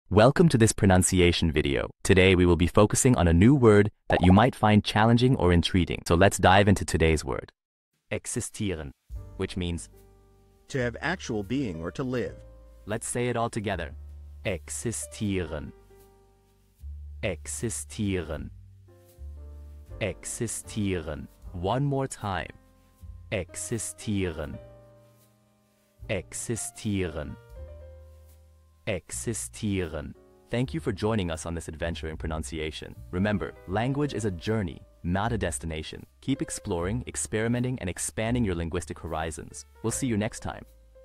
How to pronounce Existieren
Master the Pronunciation of 'Existieren which means exist' - which means : To have actual sting or to live. 📝 with @PronunciationGerman 🎓🗣️ - [Your Guide to German]

Learn to pronounce 'Existieren' perfectly in German with our easy and engaging tutorial by @PronunciationGerman! 📖📽️ In this comprehensive video, we provide you with native speaker examples 🗣️👂 and phonetic breakdowns 🎵 to ensure you get the pronunciation just right.

How to say Exist in German